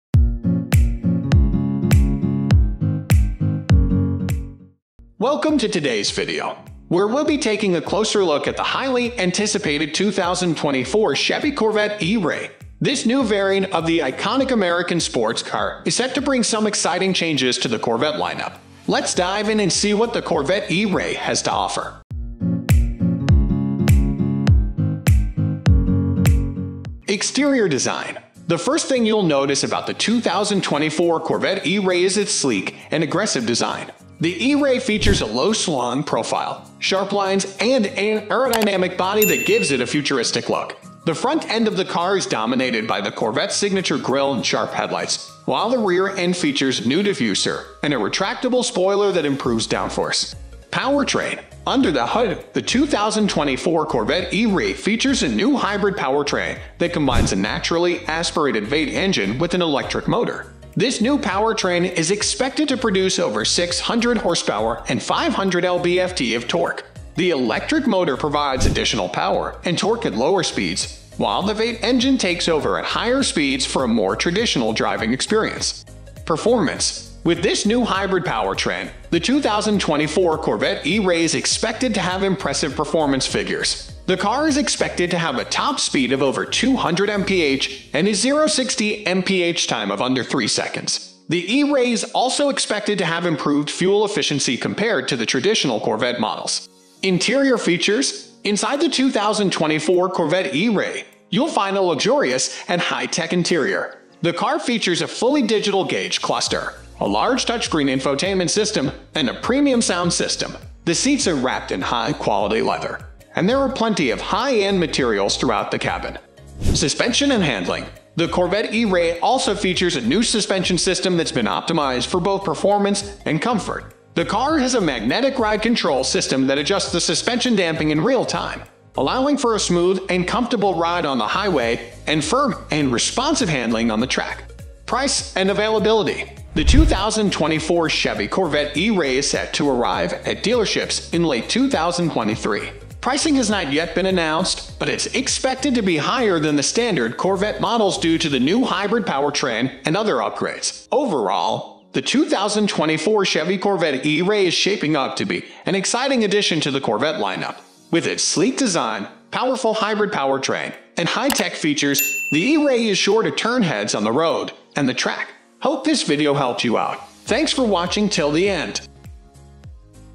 2024 Chevy Corvette E Ray Review: Unveiling the Futuristic Electric Supercar!
Welcome to our in-depth review of the highly anticipated 2024 Chevy Corvette E Ray! Join us as we explore the innovative features and exhilarating performance of this futuristic electric supercar.

🔌🚗 Looking to learn more about the 2024 Corvette E Ray? You've come to the right place! In this video, we dive into the cutting-edge technology, sleek design, and jaw-dropping speed of Chevy's first-ever electric Corvette. Get ready to witness the future of automotive engineering!

🌟 From its aerodynamic body to its state-of-the-art electric drivetrain, the 2024 Corvette E Ray is redefining what it means to be a high-performance sports car. Experience the thrill of instant acceleration, navigate sharp corners with precision, and revel in the seamless integration of advanced features.

🔋⚡ Powered by a robust electric powertrain, the Corvette E Ray boasts impressive range and lightning-fast charging capabilities. Say goodbye to range anxiety and hello to long drives filled with adrenaline and eco-consciousness.

📸 Join us on a virtual tour as we examine every detail of the Corvette E Ray's stunning interior. From luxurious materials to advanced connectivity options, Chevy has spared no expense in creating a cockpit that caters to both driver and passenger.

🔧💪 Underneath the hood, the Corvette E Ray houses cutting-edge engineering that blends performance and efficiency seamlessly. We'll explore the intricate mechanics and delve into the engineering marvels that make this electric supercar a true standout.

⏱️🆚 Are you curious about how the 2024 Corvette E Ray stacks up against its gasoline-powered counterparts?

Whether you're an automotive enthusiast, a tech aficionado, or simply curious about the future of electric sports cars, this review is a must-watch! Join us as we uncover the electrifying capabilities of the 2024 Chevy Corvette E Ray.

🔎🛒 Looking to buy a 2024 Chevy Corvette E Ray? Check out our review to make an informed decision and find the best deals near you. Don't miss your chance to own the epitome of automotive innovation!